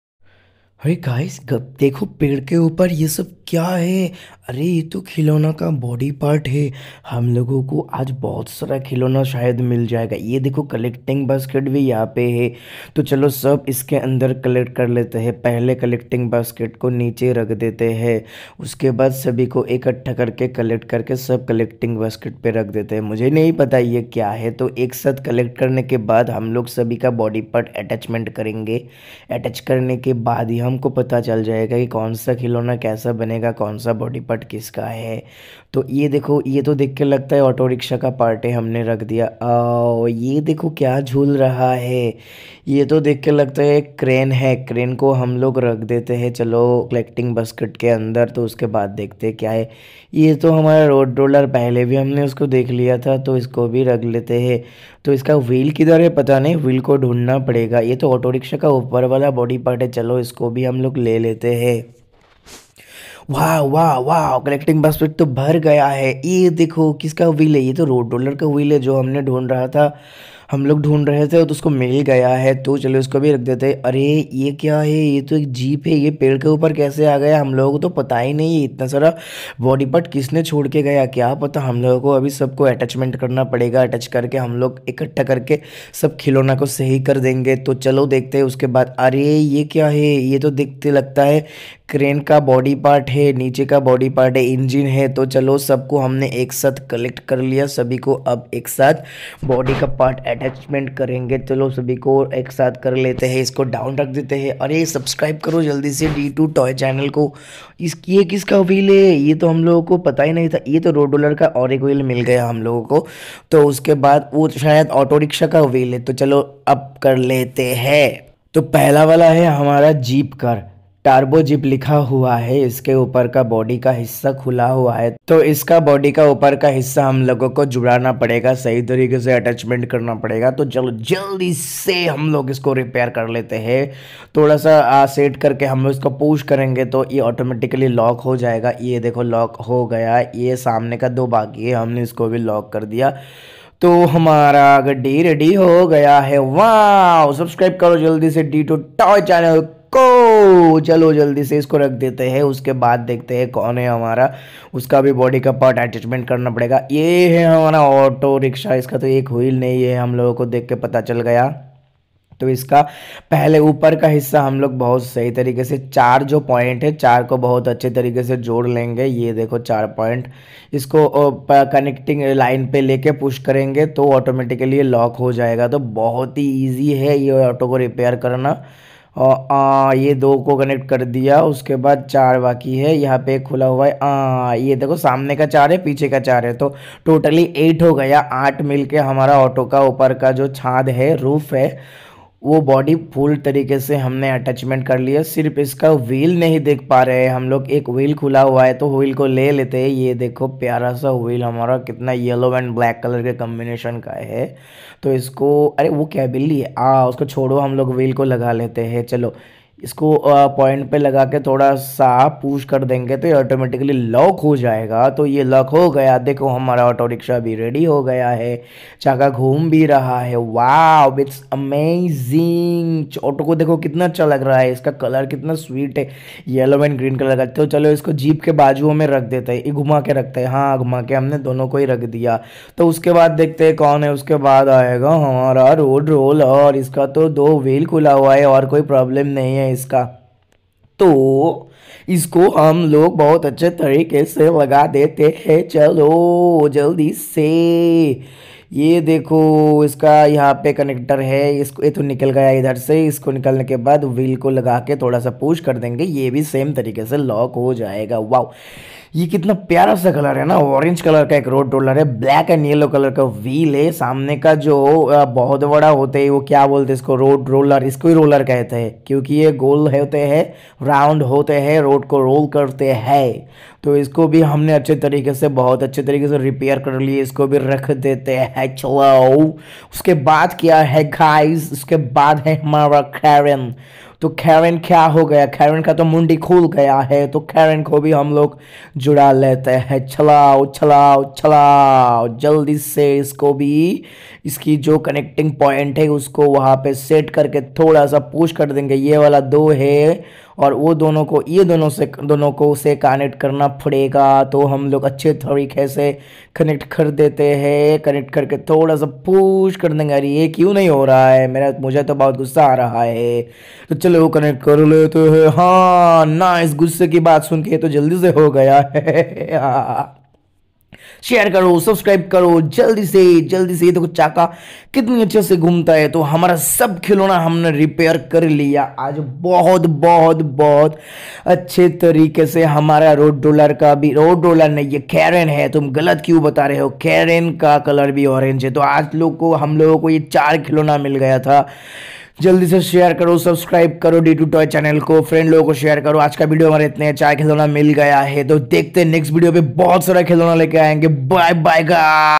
In the Ground auto rickshaw, truck, roller, tipper truck body parts finding attache | tractor toy
This iS Debobrato Biswas.
Daily new toy video update channel D2 TOY.